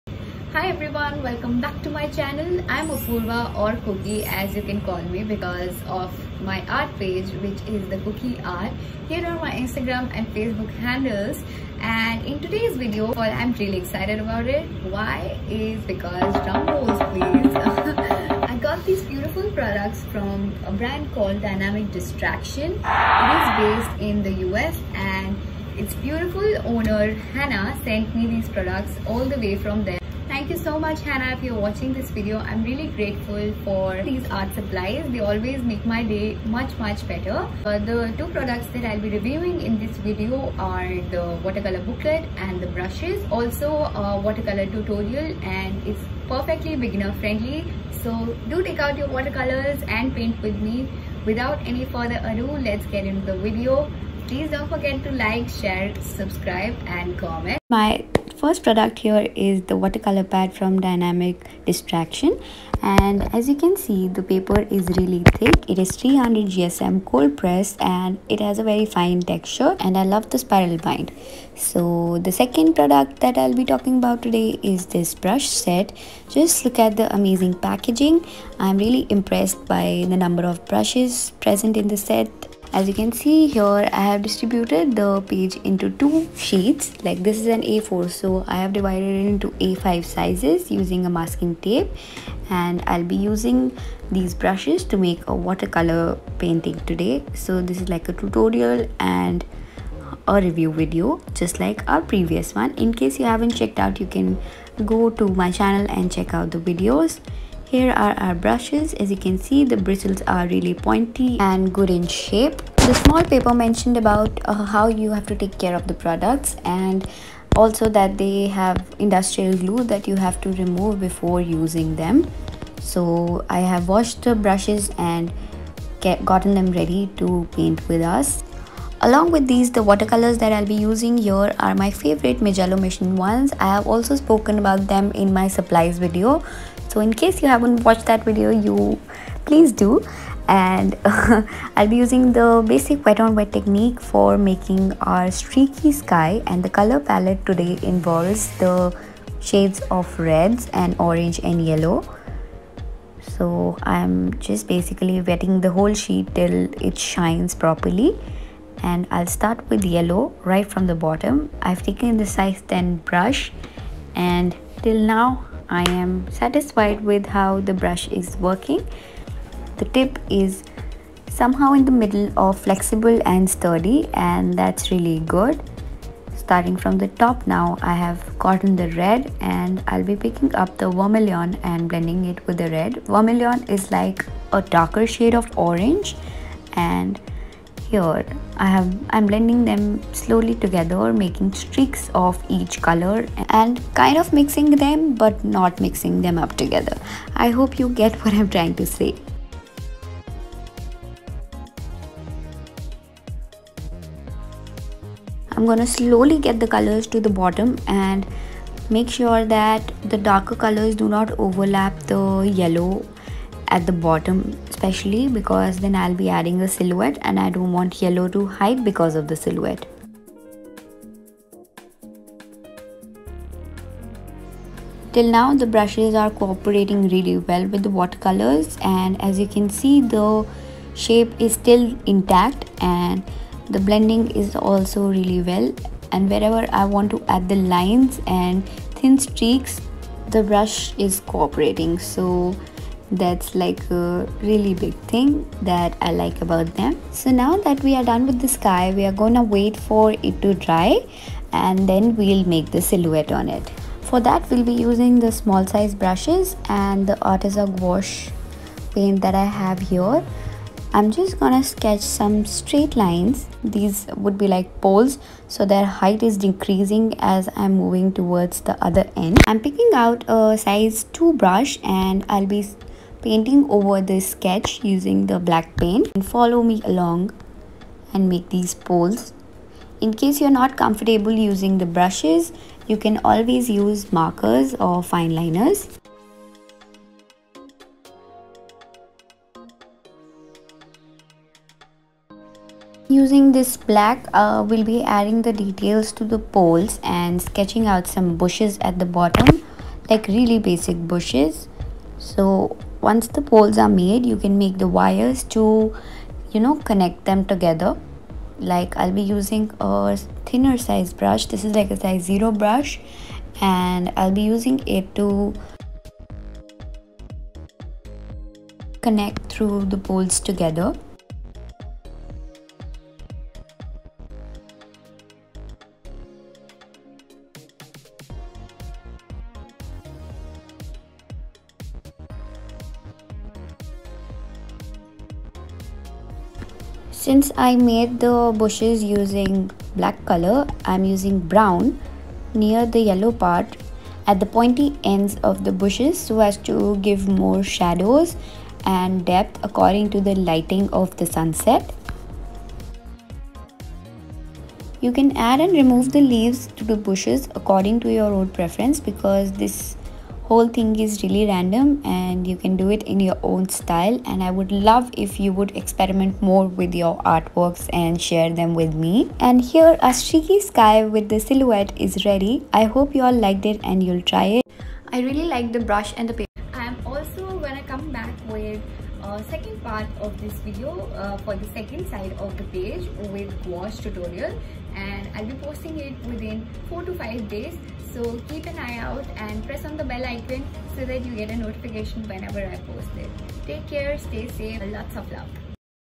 Watercolor Tutorial using Dynamic Distraction products | Easy Watercolor Painting for Beginners ✨
Watercolors with Cookie ~ 🎨

Helloooo all! ❤️
Thank you for tuning in and welcome to my happy place. I hope this channel helps you in understanding everything artsy and joyful. Happy watching! ✨

Sharing my reviews about these beautiful art products from Dynamic Distraction and a step by step Watercolor painting. I hope this video helps and don’t forget to share your results with me, please 🤗

I completely recommend the brushes and the watercolor pad as they performed really well!